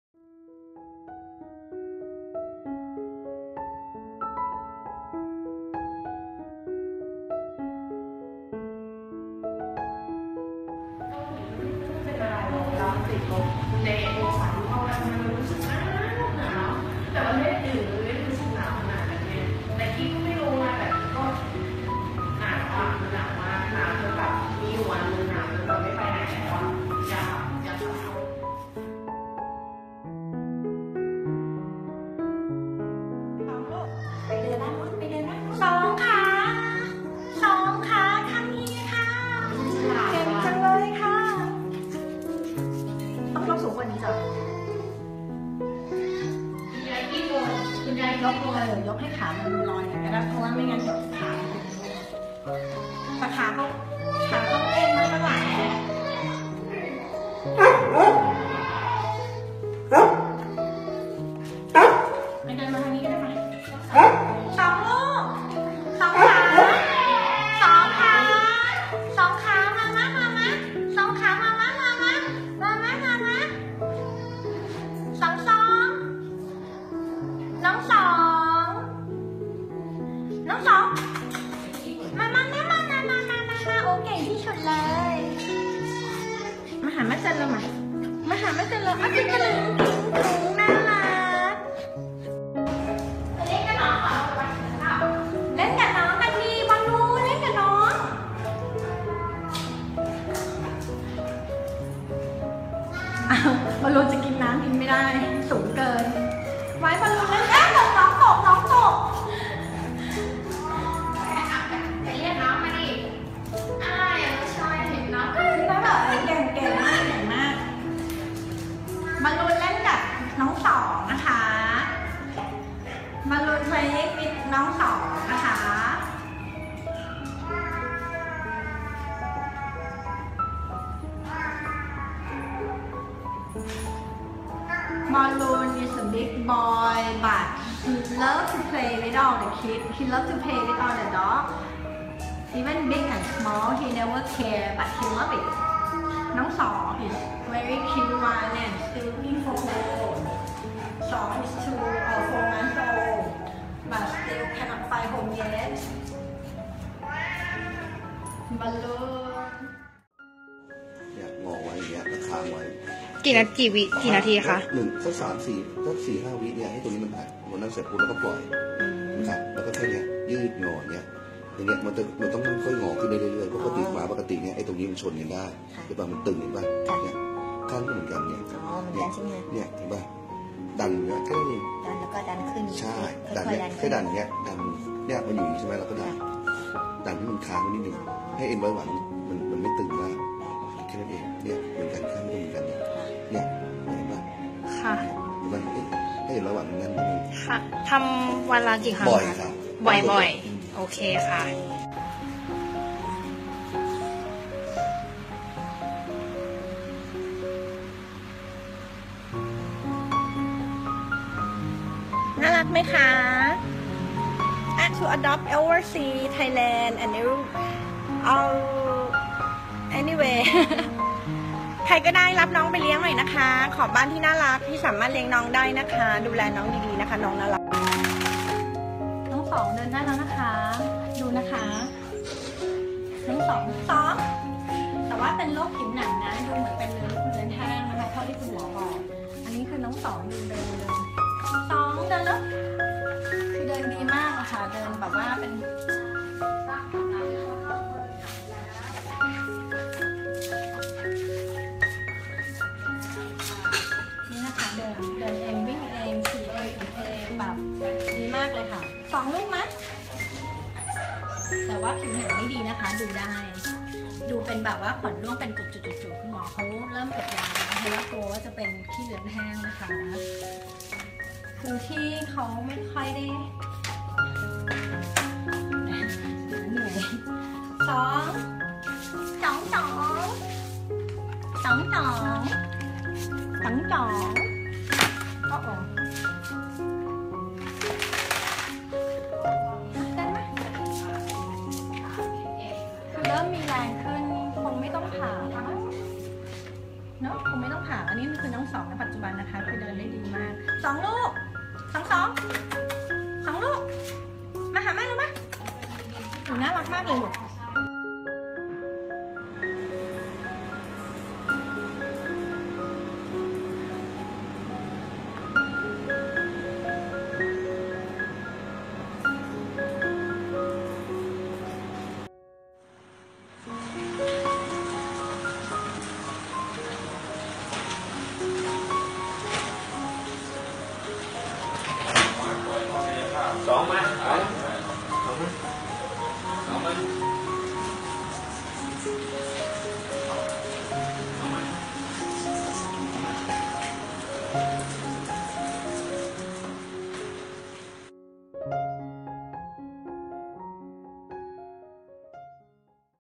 Paralyzed Puppy Underwent Amazing Physical Therapy that Helps Her Walk Again
She is taken care of by Patarin Phadungpisuth. Finally, Song can walk but it's not 100%. She is improving a lot day and day. She didn't need a spine surgery. Song is a fighter, from being paralyzed to being a normal dog again. Huge thanks for every support and every donation in giving Little Song a blissful life.

Patarin Phadungpisuth
For the love of animals. Please pass it on!